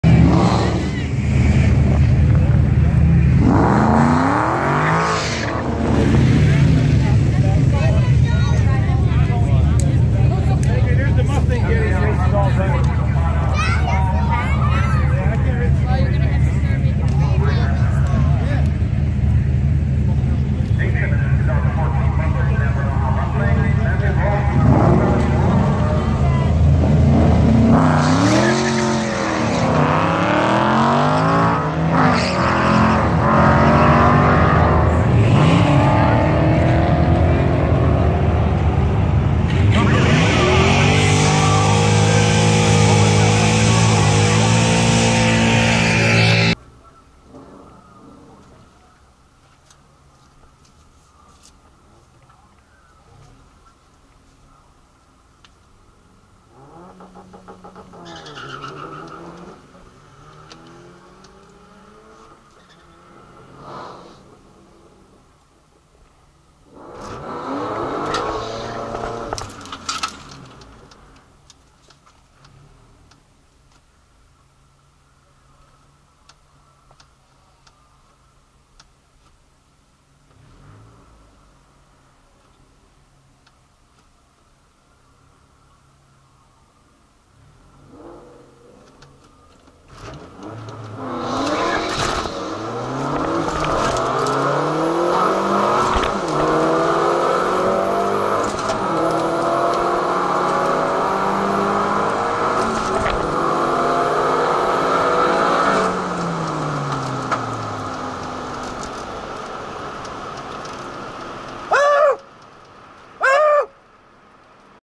mazdaspeed3 gt3076r vs 5.0 mustang with spectate/pov racing
Finally after 2 years of no track time to a new setup I beat my personal best and I did not change suspension tire pressure or anything...I ran a 13.3 AND 13.4...I NEED SLICKS DAMN IT. enjoy